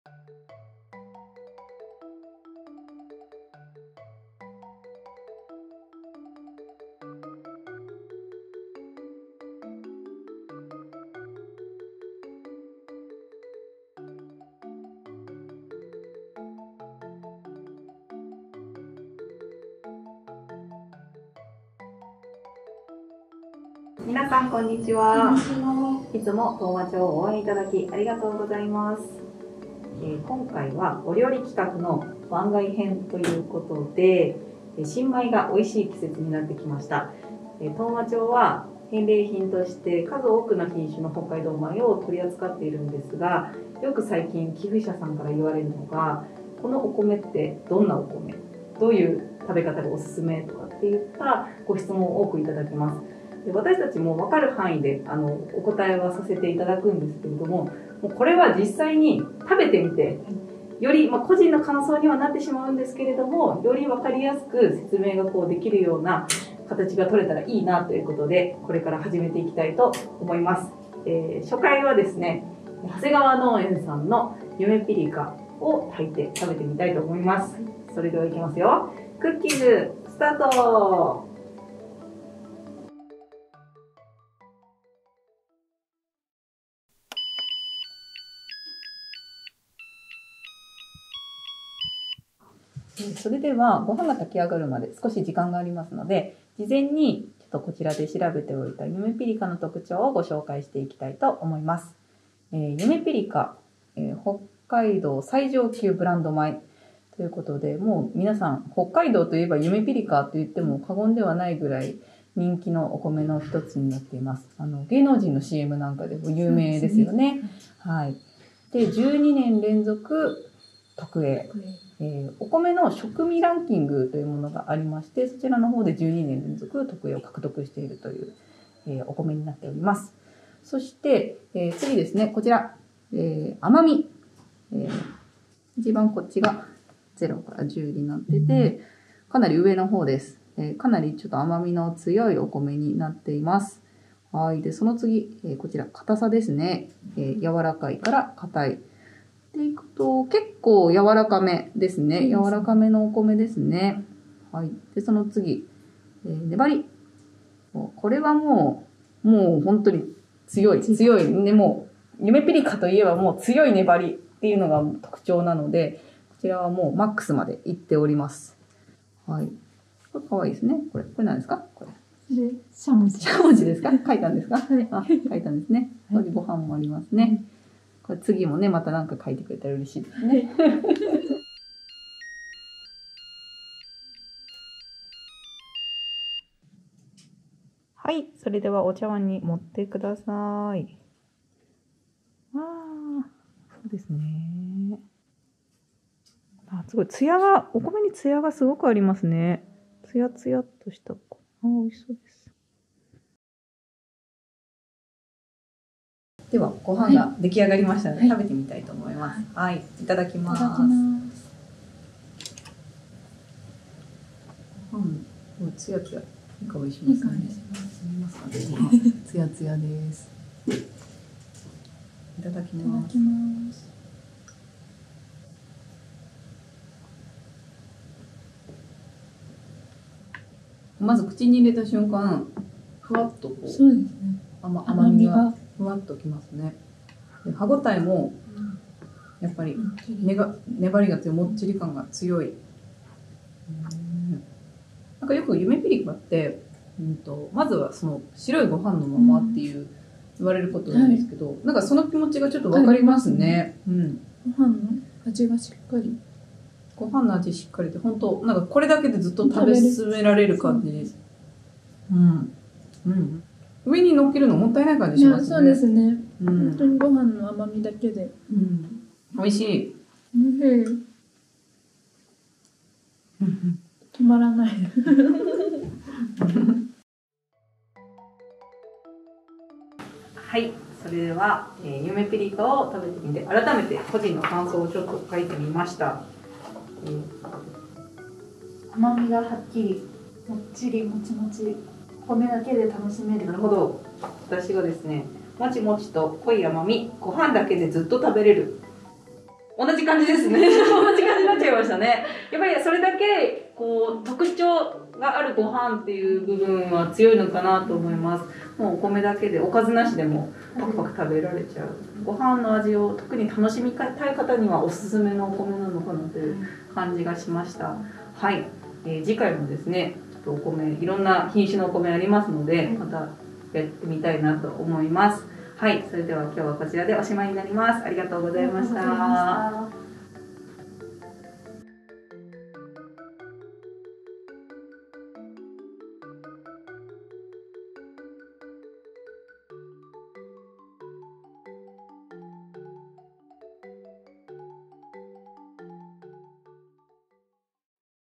これは美味い！ゆめぴりか！
さまざまな品種のお米を提供している当麻町のふるさと納税。今回は北海道のブランド米として知られる｢ゆめぴりか｣を食べてみました！
【当麻町のふるさと納税はこちらから】

【当麻町のおすすめふるさと納税返礼品】